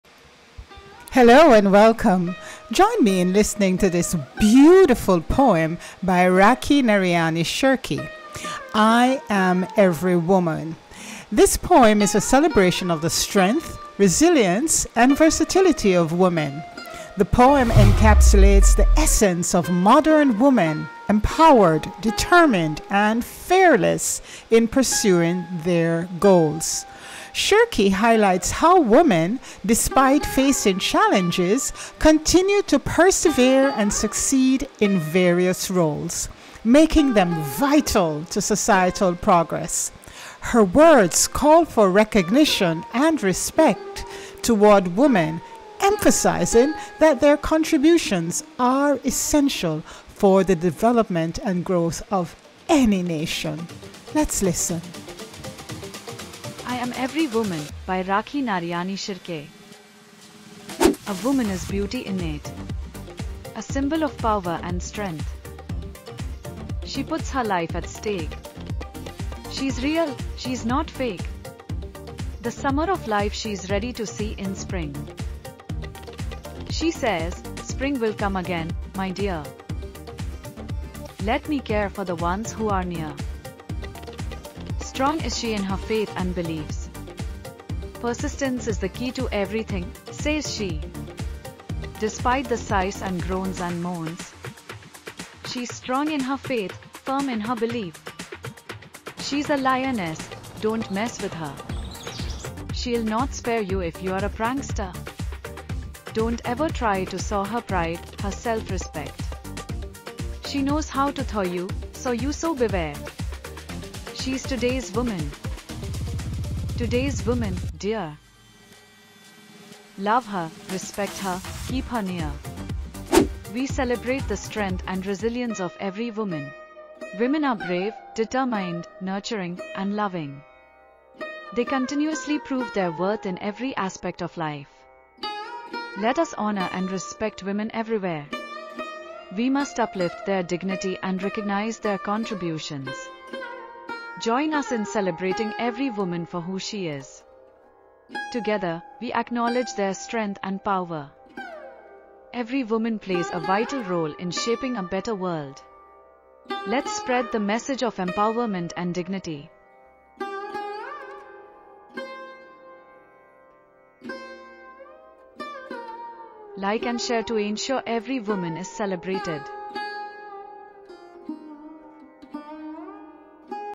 Top Female Poets Reveal the Strength of Modern Women
"I Am Every Woman," a poignant poem by Rakhi Naraini Shrike makes material for a video that delves into the powerful themes that resonate with modern women and explores the essence of womanhood. This literary review takes you through the intricate tapestry of feminine power, artistic expression, and the strength found in femininity.

As we discuss the memory poem in English, particularly relevant for students in the 10th standard, we highlight how it reflects the experiences and challenges faced by women today, especially those over 40. Join us as we celebrate women’s empowerment and analyze the themes of equality and strength in femininity woven throughout this remarkable piece.

Whether you're a literature enthusiast or simply interested in the poetic themes of modern womanhood, this video offers insights that connect to the broader narrative of women's literature and the ongoing journey toward empowerment. Tune in and discover how "I Am Every Woman" serves as a powerful anthem for all women, showcasing the resilience and unity that define us.